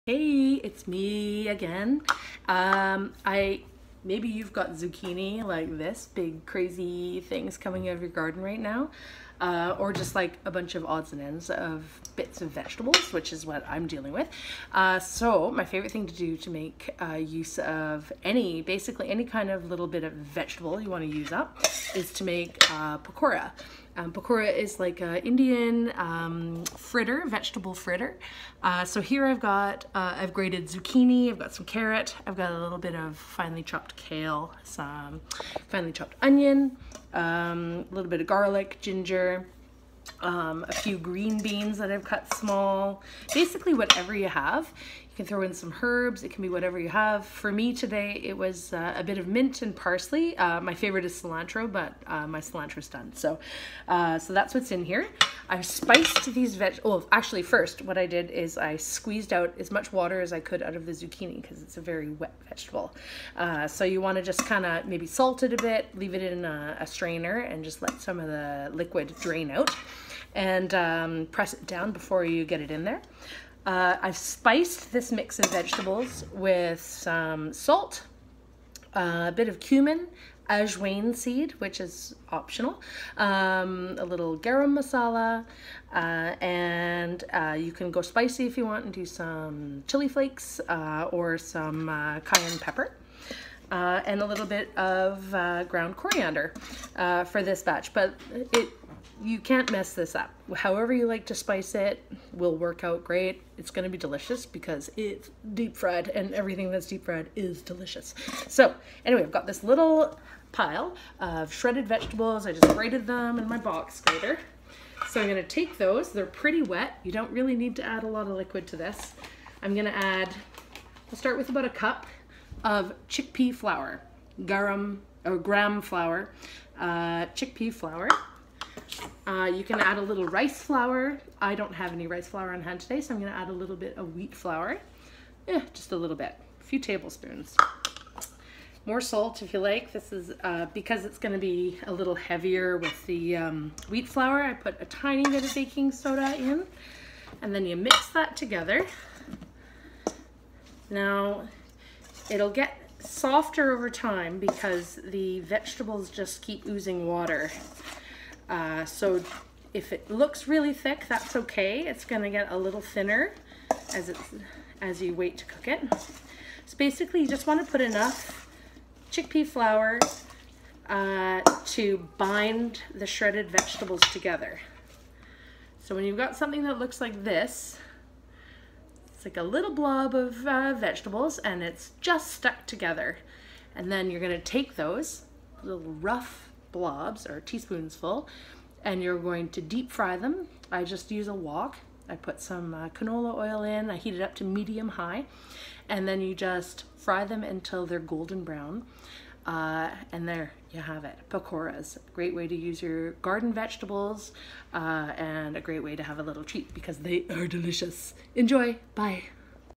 Vegetable pakora
Clean out your crisper or use that giant zucchini to make pakora fritters